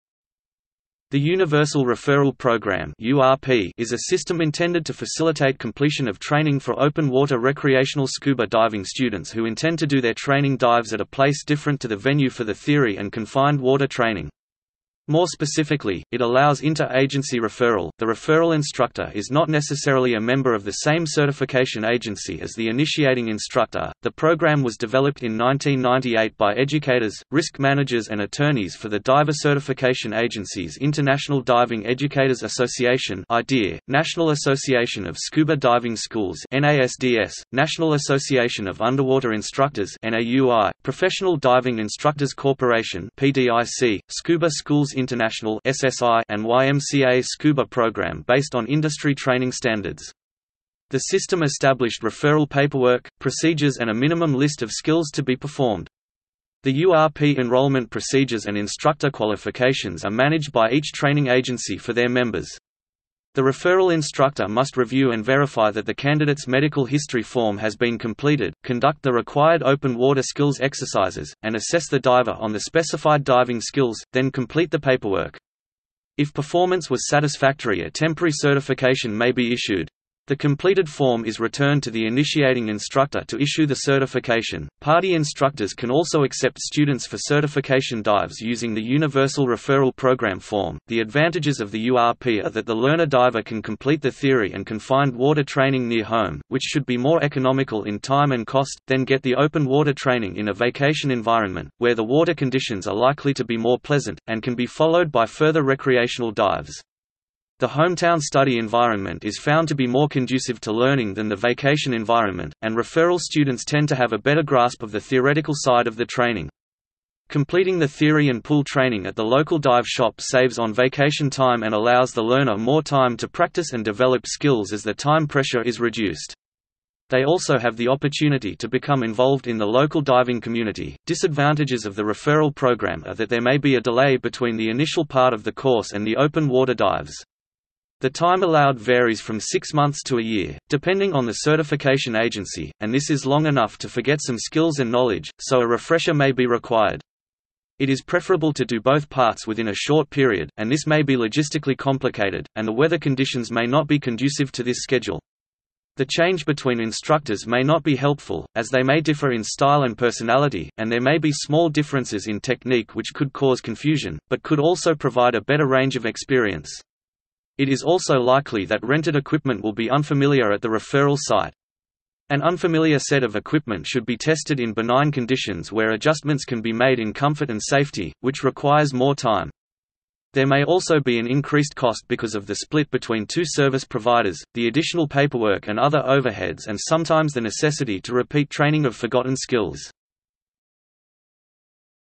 Universal Referral Program | Wikipedia audio article
- Socrates

SUMMARY
The Universal Referral Program (URP) is a system intended to facilitate completion of training for open water recreational scuba diving students who intend to do their training dives at a place different to the venue for the theory and confined water training. More specifically, it allows inter agency referral - the referral instructor is not necessarily a member of the same certification agency as the initiating instructor.The program was developed in 1998 by educators, risk managers and attorneys for the diver certification agencies International Diving Educators Association (IDEA), National Association of Scuba Diving Schools (NASDS), National Association of Underwater Instructors (NAUI), Professional Diving Instructors Corporation (PDIC), Scuba Schools International (SSI) and YMCA SCUBA Program based on industry training standards. The system established referral paperwork, procedures and a minimum list of  skills to be performed. The URP enrollment procedures and instructor qualifications are managed by each training agency for their members. The referral instructor must review and verify that the candidate's medical history form has been completed, conduct the required open water skills exercises, and assess the diver on the specified diving skills, then complete the paperwork. If performance was satisfactory a temporary certification may be issued. The completed form is returned to the initiating instructor to issue the certification.PADI instructors can also accept students for certification dives using the Universal Referral Program form.The advantages of the URP are that the learner diver can complete the theory and confined water training near home, which should be more economical in time and cost, then get the open water training in a vacation environment, where the water conditions are likely to be more pleasant, and can be followed by further recreational dives. The hometown study environment is found to be more conducive to learning than the vacation environment, and referral students tend to have a better grasp of the theoretical side of the training. Completing the theory and pool training at the local dive shop saves on vacation time and allows the learner more time to practice and develop skills as the time pressure is reduced. They also have the opportunity to become involved in the local diving community.Disadvantages of the Referral Program are that there may be a delay between the initial part of the course and the open water dives. The time allowed varies from 6 months to a year, depending on the certification agency, and this is long enough to forget some skills and knowledge, so a refresher may be required. It is preferable to do both parts within a short period, and this may be logistically complicated, and the weather conditions may not be conducive to this schedule. The change between instructors may not be helpful, as they may differ in style and personality, and there may be small differences in technique which could cause confusion, but could also provide a better range of experience. It is also likely that rented equipment will be unfamiliar at the referral site. An unfamiliar set of equipment should be tested in benign conditions where adjustments can be made in comfort and safety, which requires more time. There may also be an increased cost because of the split between two service providers, the additional paperwork and other overheads and sometimes the necessity to repeat training of forgotten skills.